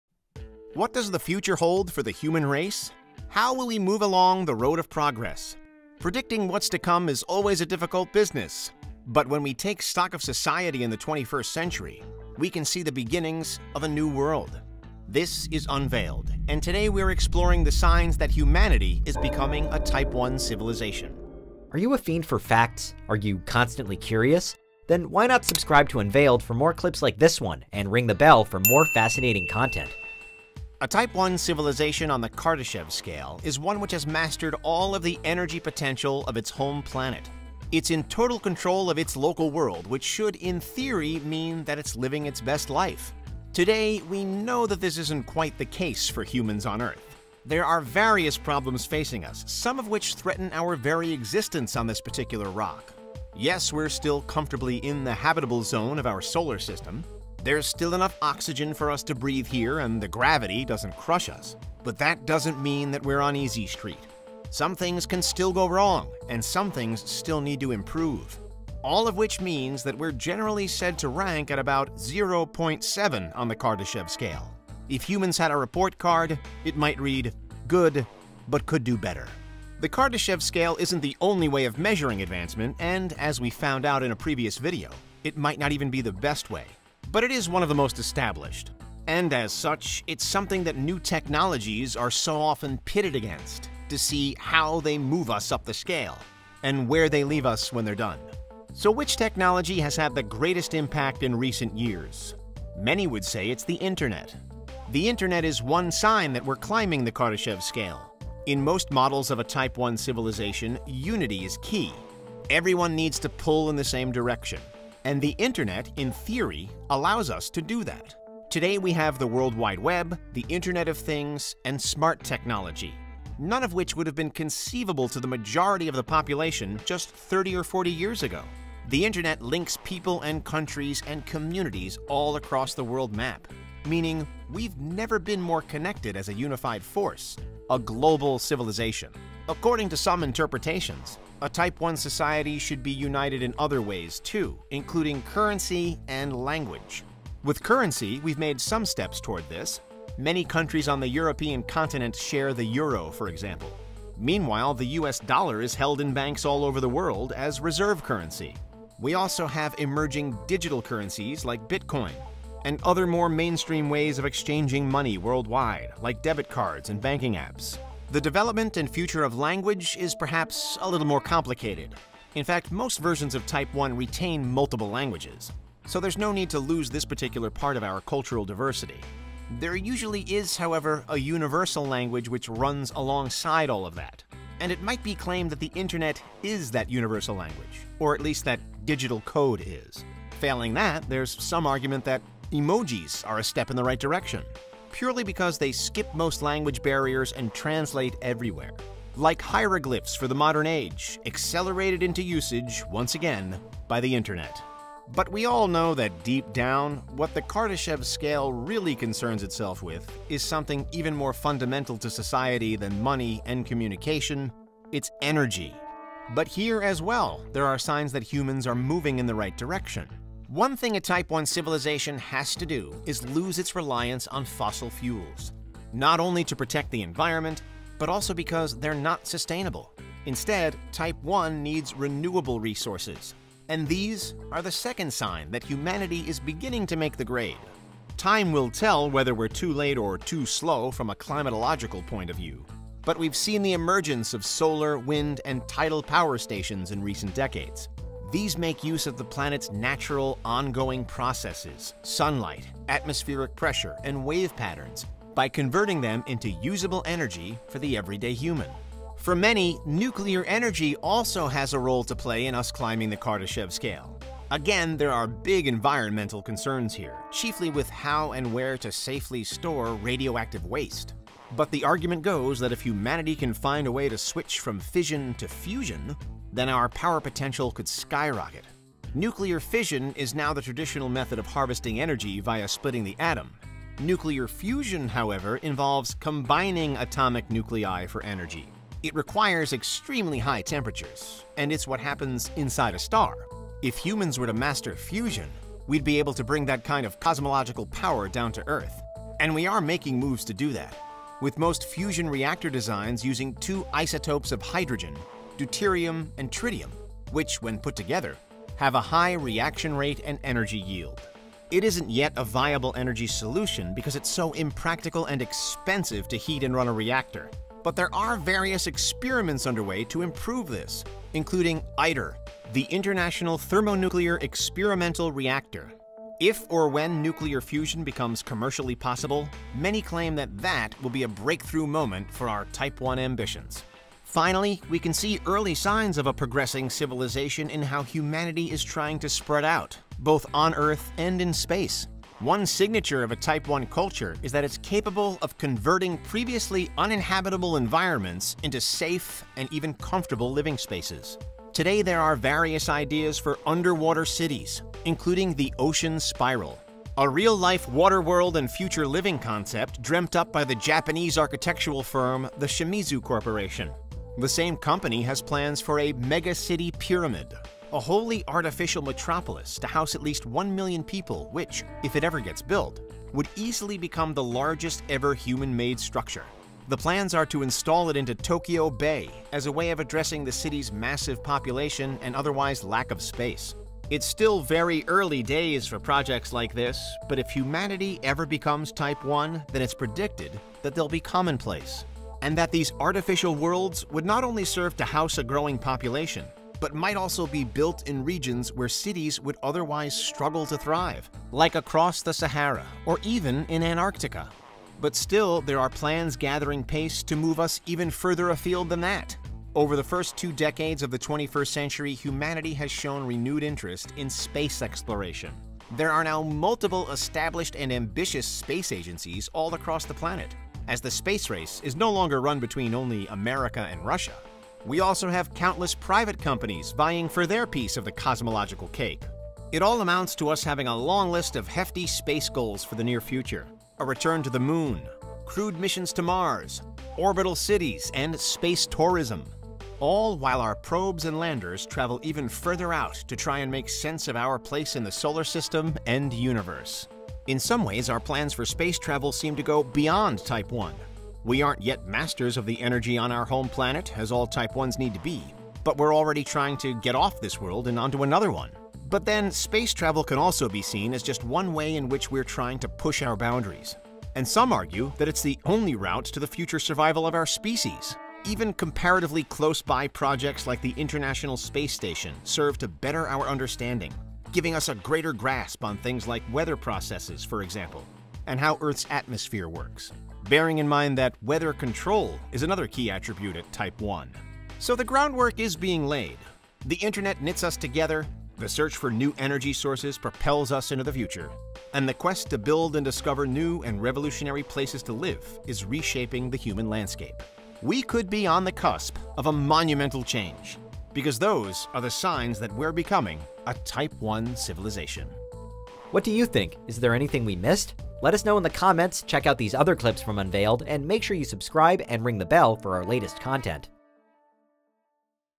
3 Signs We're Becoming a Type I Civilization | Unveiled
How close are humans to becoming a Type I Civilization? Join us... and find out!

In the 1970s, Carl Sagan famously calculated that humankind ranked at just 0.7 on the Kardashev Scale. In this video, we investigate how much we've improved in the years since then. How have we advanced? And where do we now rank on Kardashev's famous system? We're closer than ever to breaking into Type I... but what still needs to be done?